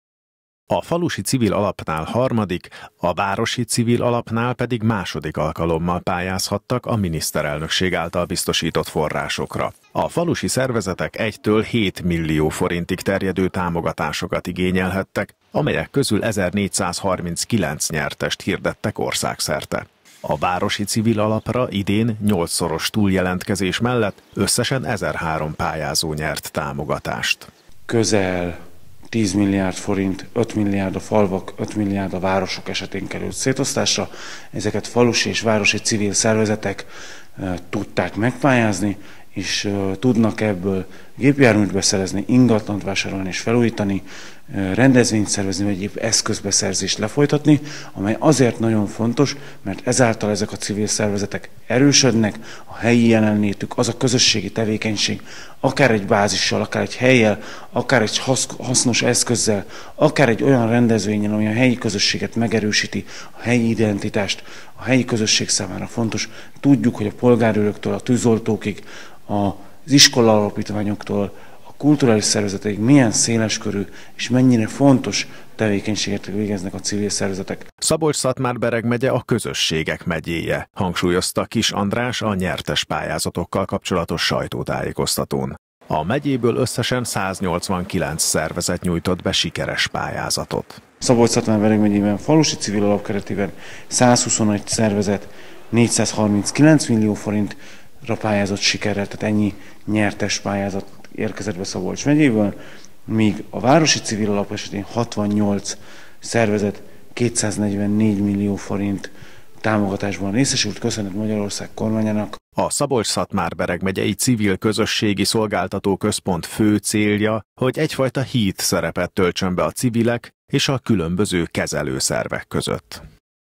Civil támogatások
Összesen 189 civil szervezet nyújtott be sikeres pályázatot a Falusi és a Városi Civil Alaphoz Szabolcs-Szatmár-Bereg megyéből az elmúlt pályázati ciklusban. A falusi civil szervezetek összesen 439 millió forint, a városi civilek pedig 244 millió forint támogatásban részesülnek.